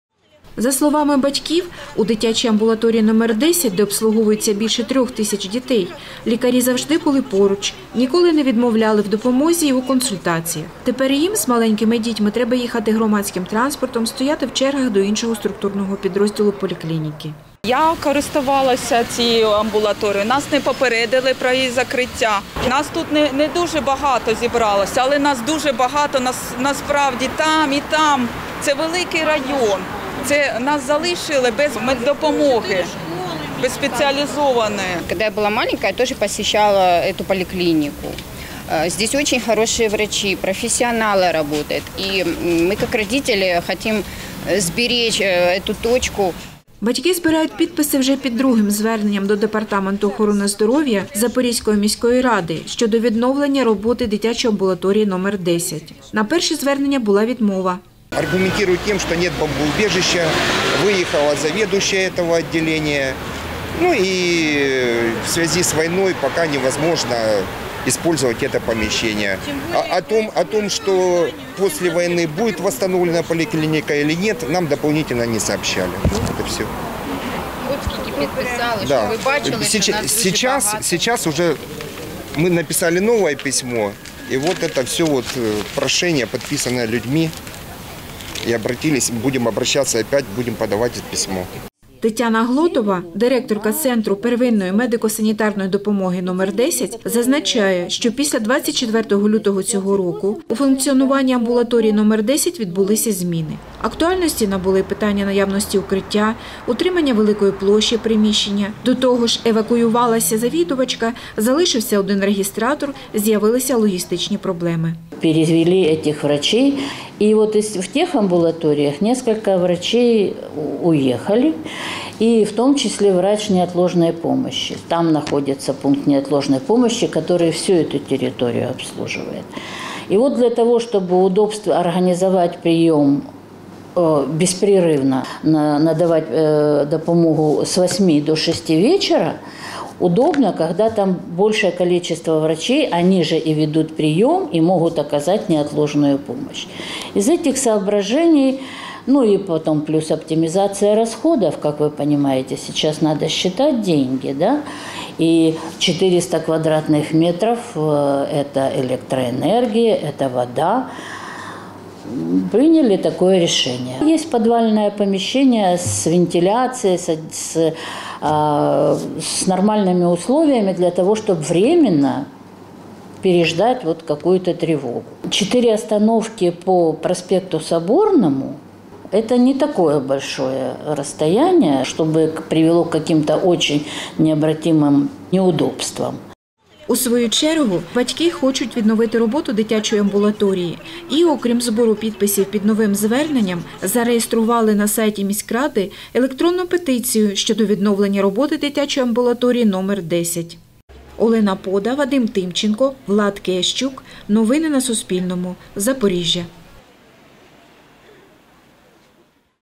У Запоріжжі закрили дитячу амбулаторію. Батьки дітей збирають підписи проти закриття | Новини
Амбулаторію № 10 Центру первинної медико-санітарної допомоги № 10, що в Дніпровському районі Запоріжжя, закрито. Наразі сімейні дитячі лікарі приймають своїх пацієнтів в іншому структурному підрозділі цього ж Центру. Батьки з таким рішенням департаменту охорони здоров’я Запорізької міської ради не погоджуються.